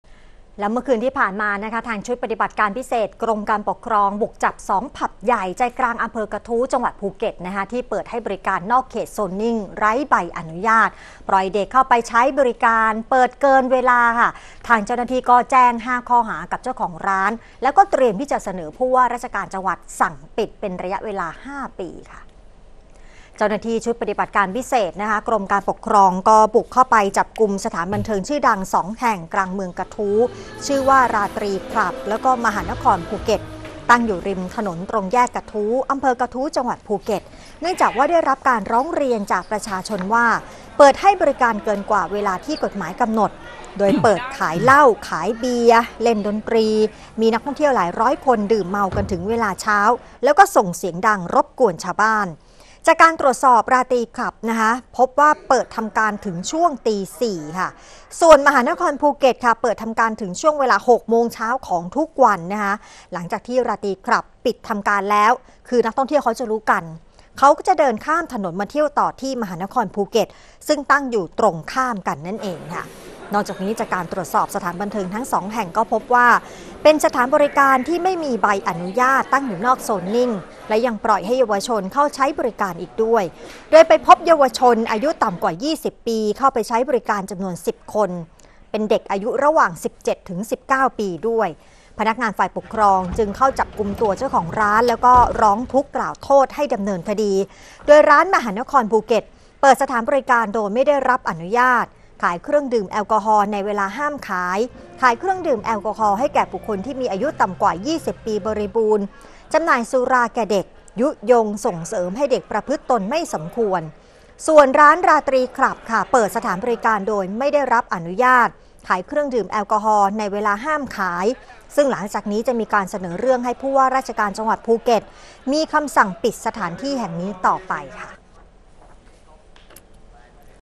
ชุดปฏิบัติการพิเศษ บุกจับ 2 ผับใหญ่ จ.ภูเก็ต ข่าวเที่ยง วันที่ 1 มิถุนายน 2566
ชุดปฏิบัติการพิเศษกรมการปกครอง บุกจับ 2 ผับใหญ่ ใจกลางอำเภอกะทู้ จังหวัดภูเก็ต เปิดให้บริการนอกเขตโซนนิ่ง ไร้ใบอนุญาต ปล่อยเด็กเข้าใช้บริการ เปิดเกินเวลา เจ้าหน้าที่แจ้ง 5 ข้อหากับเจ้าของร้าน เตรียมเสนอผู้ว่าราชการจังหวัดสั่งปิด 5 ปี